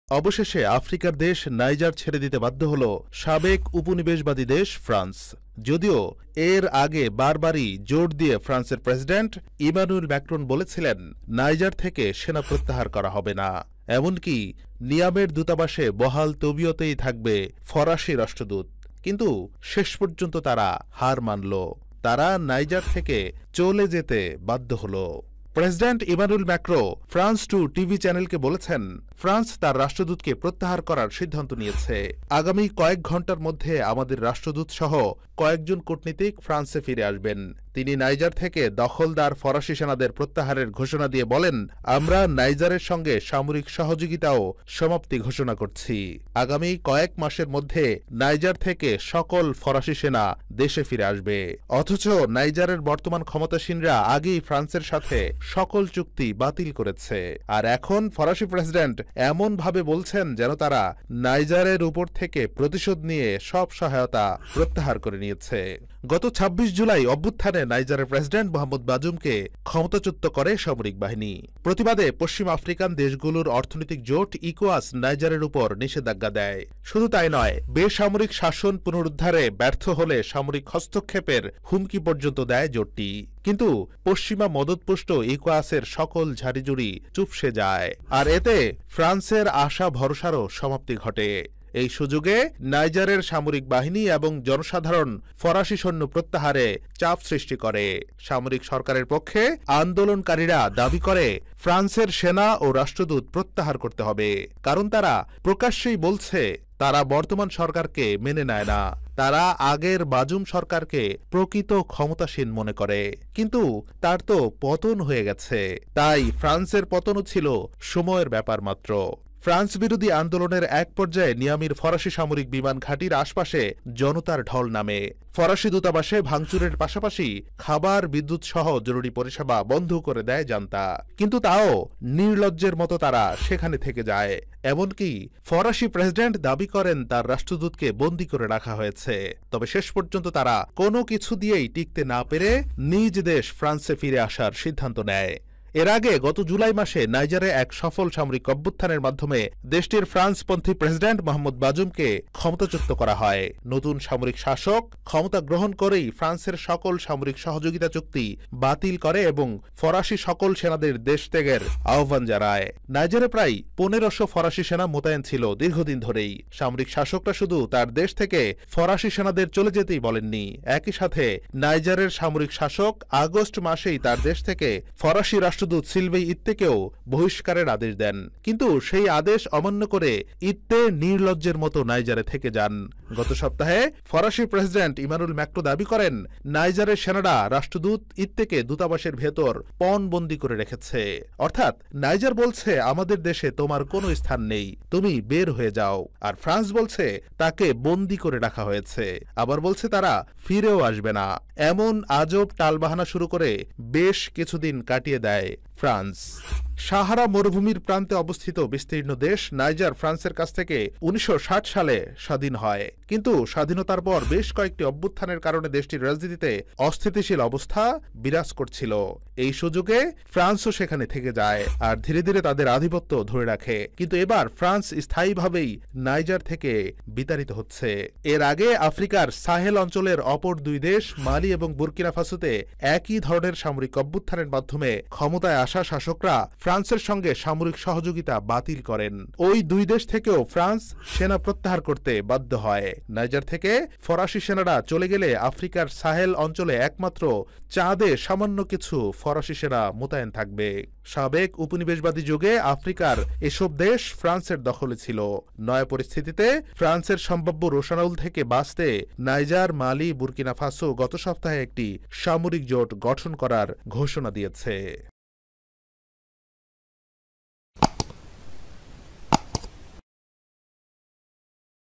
শেষ পর্যন্ত নাইজারের কাছে হার মানলো ফ্রান্স! France–Niger | News View
অবশেষে আফ্রিকার দেশ নাইজার ছেড়ে দিতে বাধ্য হলো সাবেক উপনিবেশবাদী দেশ ফ্রান্স। যদিও এর আগে বার বারই জোর দিয়ে ফরাসি প্রেসিডেন্ট ইমানুয়েল ম্যাকরন বলেছিলেন, নাইজার থেকে সেনা প্রত্যাহার করা হবে না।  এমনকি নিয়ামের দূতাবাসে বহাল তবিয়তেই থাকবে ফরাসী রাস্ট্রদূত। কিন্তু শেষ পর্যন্ত তারা হার মানলো। তারা নাইজার থেকে সেনা প্রত্যাহার করতে বাধ্য হলো ।

বিভিন্ন জেলা ও দেশ-বিদেশে সকল আপডেট পেতে আমাদের চ্যানেলটি সাবস্ক্রাইভ করুন